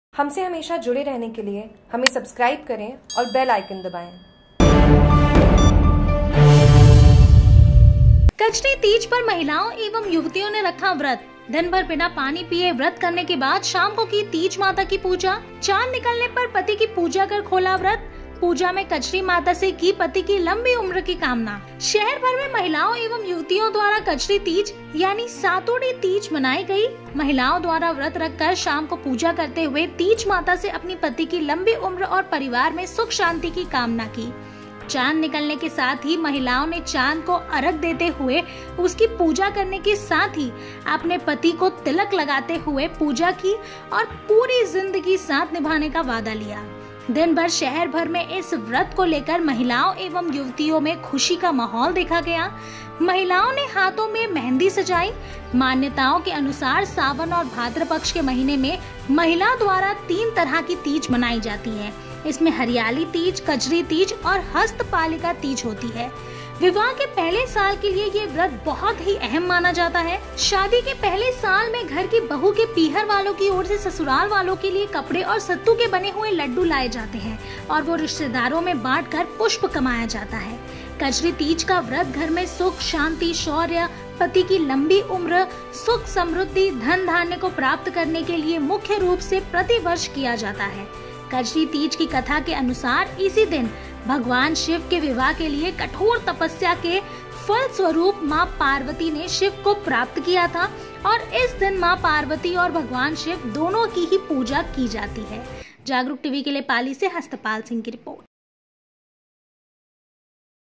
Jagruk TV पाली : कजरी तीज पर महिलाओं एवं युवतियों ने रखा व्रत
पाली

शहर भर में महिलाओं और युवतियों द्वारा कजरी तीज यानी सातुड़ी तीज मनाई गई। महिलाओं द्वारा व्रत रखकर शाम को पूजा करते हुए तीज माता से अपने पति की लंबी उम्र और परिवार में सुख शांति की कामना की।

चांद निकलने के साथ ही महिलाओं ने चांद को अरग देते हुए उसकी पूजा करने के साथ ही अपने पति को तिलक लगाते हुए पूजा की और पूरी जिंदगी साथ निभाने का वादा लिया।

दिन भर शहर भर में इस व्रत को लेकर महिलाओं और युवतियों में खुशी का माहौल देखा गया। महिलाओं ने हाथों में मेहंदी सजाई। मान्यताओं के अनुसार सावन और भाद्र पक्ष के महीने में महिला द्वारा तीन तरह की तीज मनाई जाती है इसमें हरियाली तीज, कजरी तीज और हस्त पालिका तीज होती है।

विवाह के पहले साल  के लिए यह व्रत बहुत ही अहम माना जाता है। शादी के पहले साल में घर की बहू के  पीहर वालों की ओर से ससुराल वालो के लिए कपड़े और सत्तू से बने हुए लड्डू लाए जाते हैं और वह रिश्तेदारों में बांटकर पुण्य कमाया जाता है।

कजरी तीज का व्रत घर में सुख, शांति, शौर्य, पति की लंबी उम्र, सुख समृद्धि, धन-धान्य को प्राप्त करने के लिए मुख्य रूप से प्रति वर्ष किया जाता है। कजरी तीज की कथा के अनुसार इसी दिन भगवान शिव के विवाह के लिए कठोर तपस्या के फलस्वरूप मां
पार्वती ने शिव को प्राप्त किया था और इस दिन मां पार्वती और भगवान शिव दोनों की ही पूजा की जाती हैं।

  पाली से हस्तपाल सिंह की रिपोर्ट

Jagruk TV